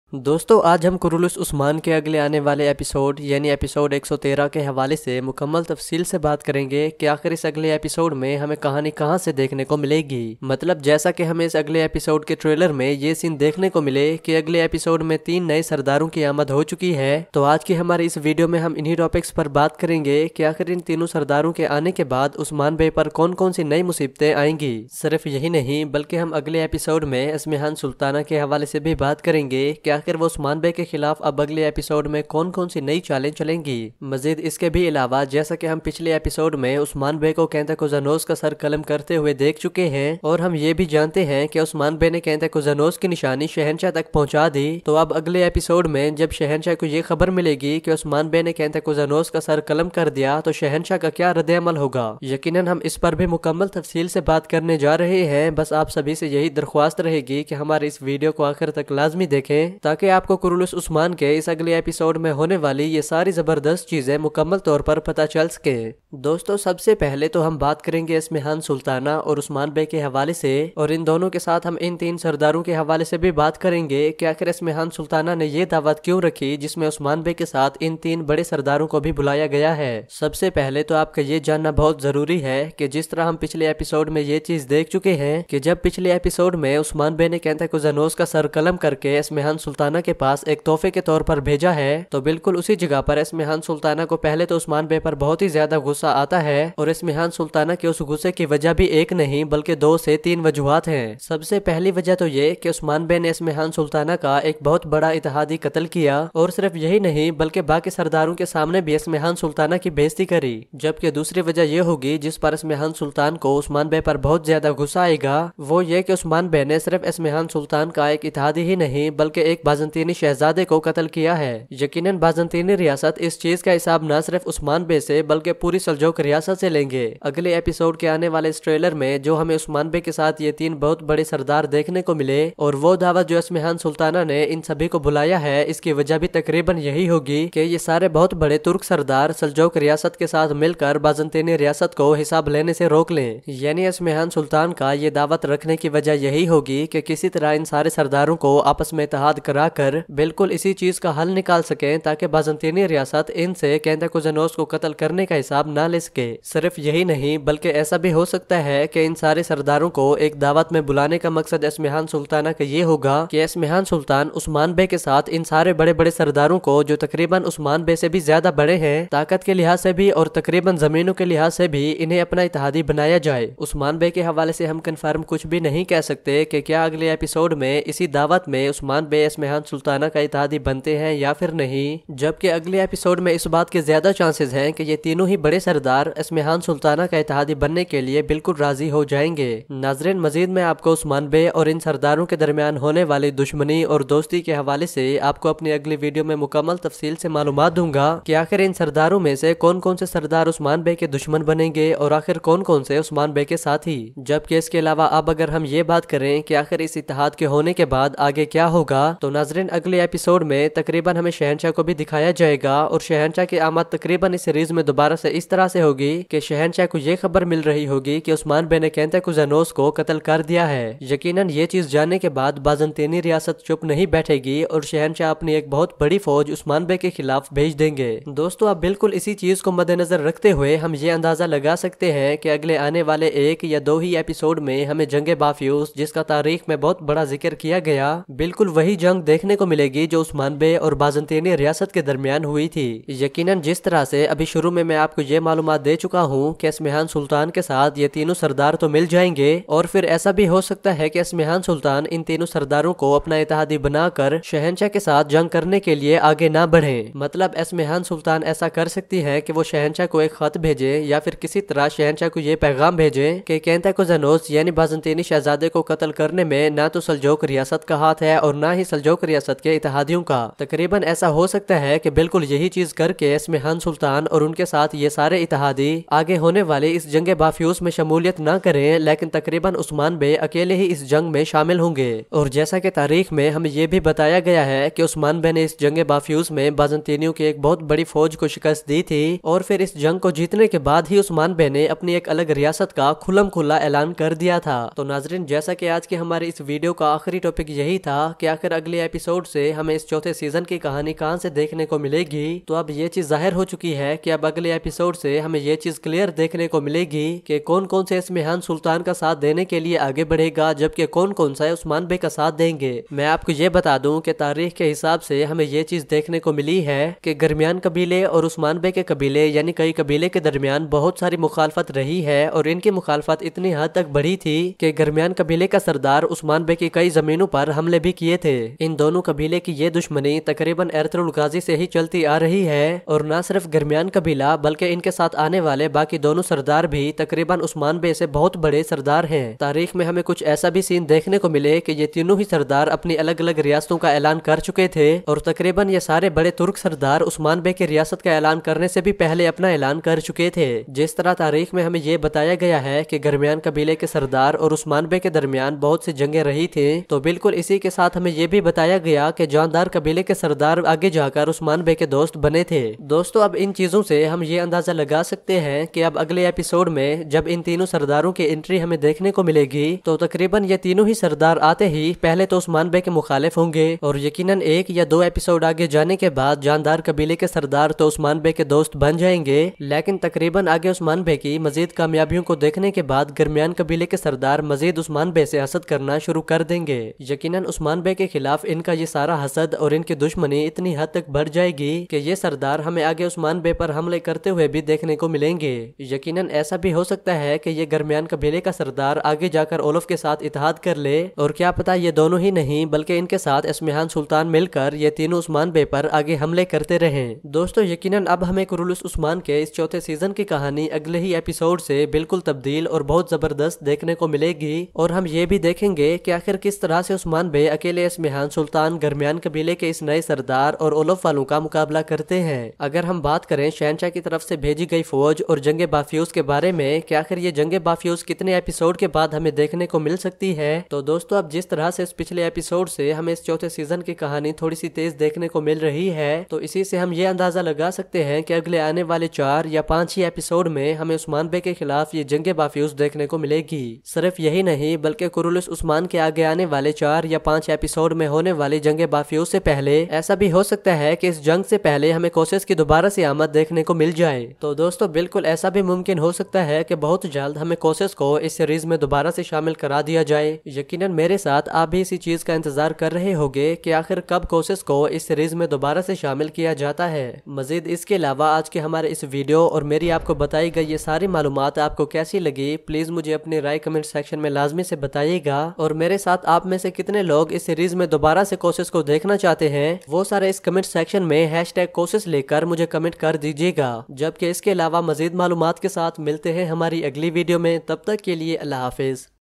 Kurulus Osman Episode 113 Trailer 2 Review In Urdu | Osman Bey Vs Esmihan Sultana | Ghazi Empire
Hye guys ! Here i'm gonna tell you Some Important Information About KurulusOsman Episode 113 Trailer 2 Review In Urdu | OsmanBey Vs Esmihan Sultana